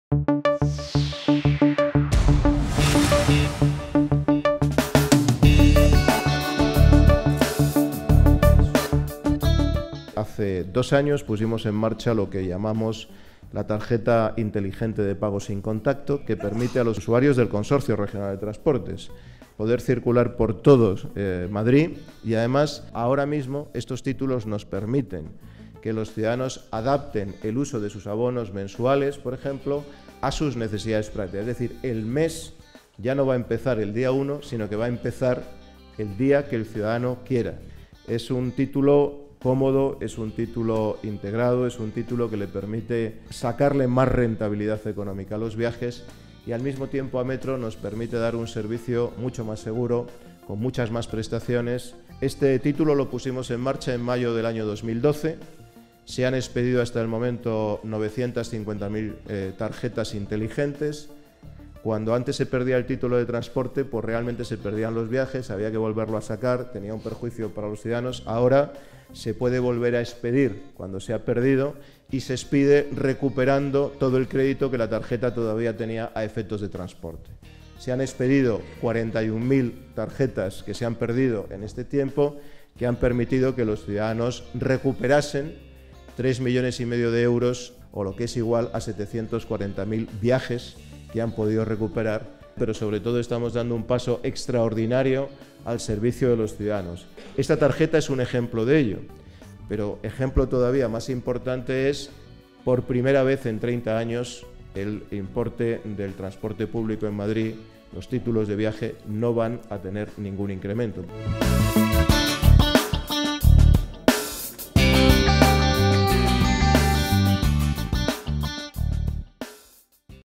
Ignacio González visita el centro de funcionamiento de las nuevas tarjetas de transporte
El Presidente de la Comunidad de Madrid, Ignacio González, visita el Centro de Desarrollo y Conformidad de Tecnología sin Contacto para Transporte Público (CDC) del Consorcio Regional de Transportes de Madrid, en el que se realizan las pruebas de funcionamiento de las nuevas tarjetas de transporte público, que ya han sustituido al abono magnético en toda la zona A y próximamente lo hará a la zona B.